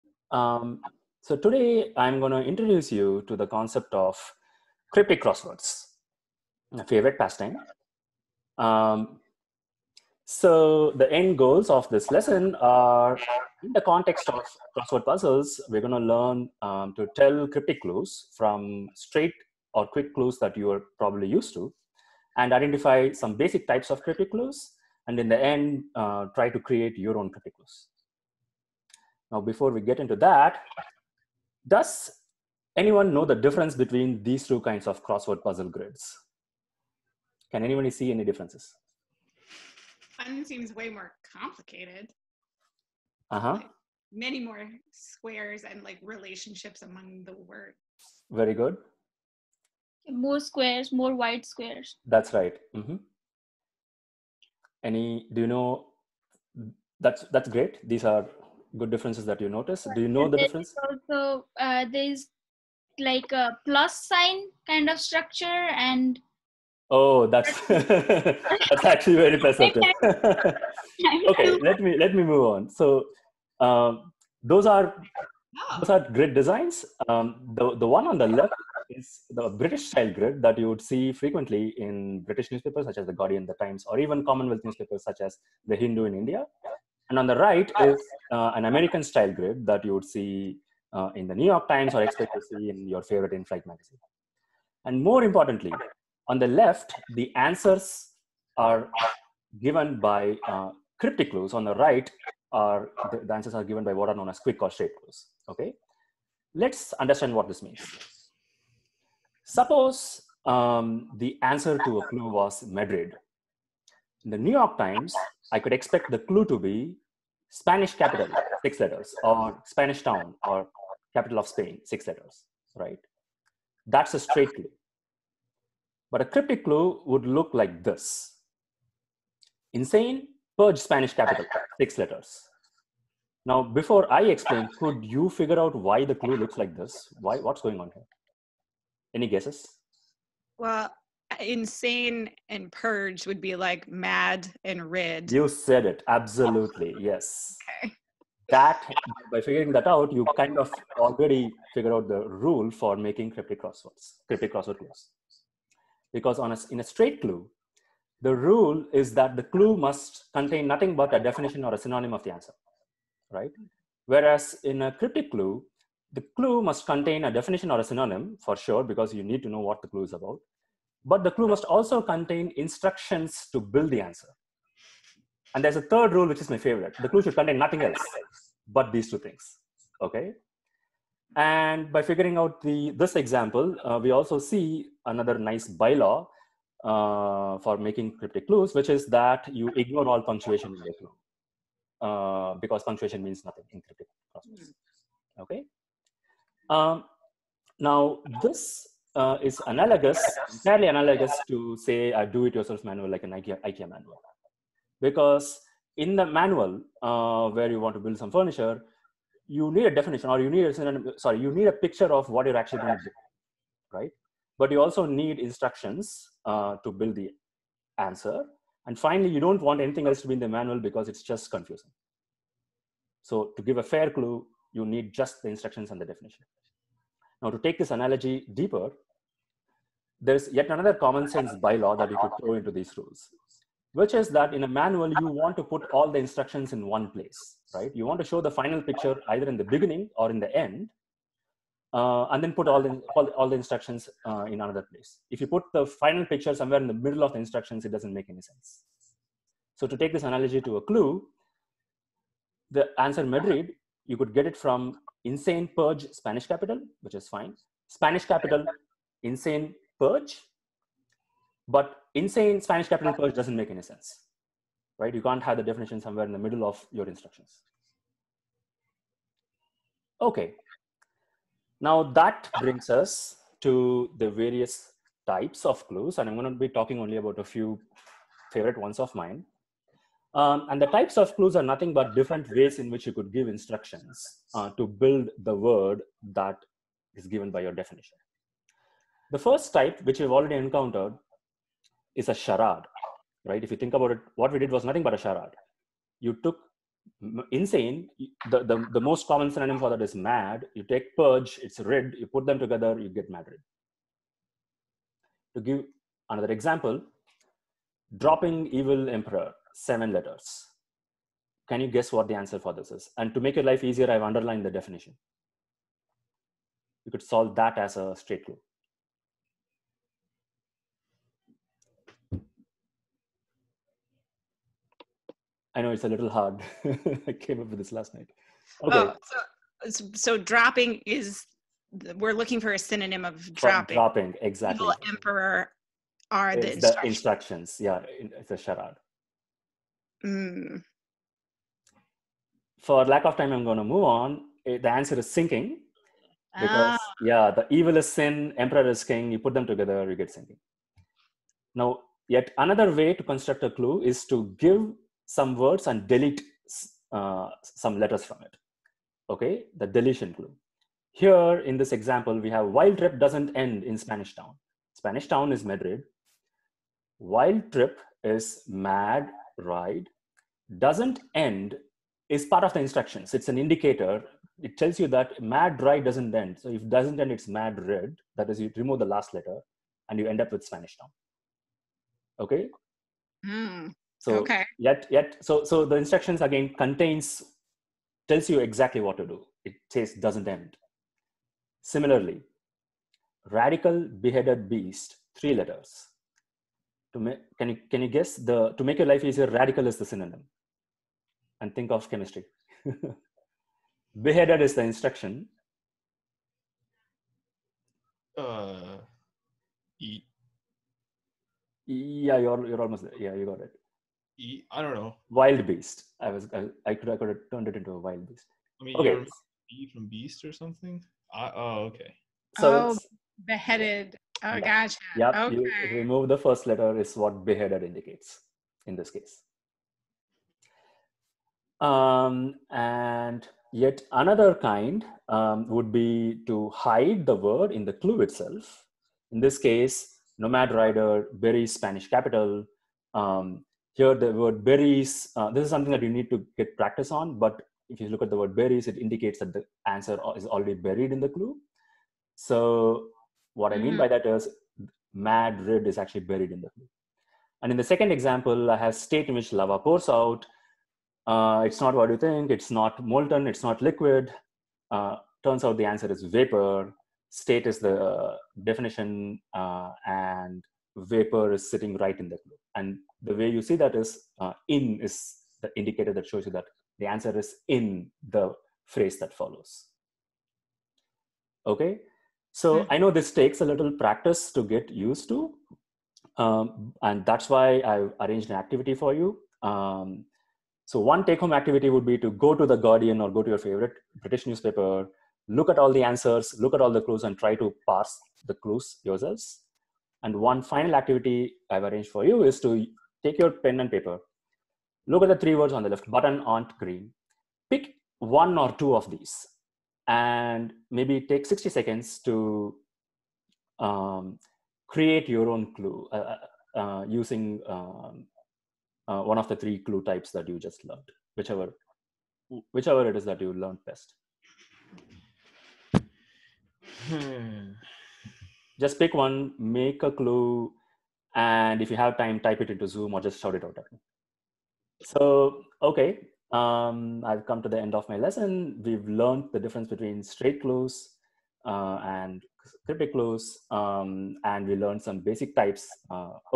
Instructional Skills Workshop @ UBC : Mini-lesson 03 : Cryptic Crosswords
Cryptic versus straight crossword clues, the rules of constructing a cryptic clue, and three ways to construct one.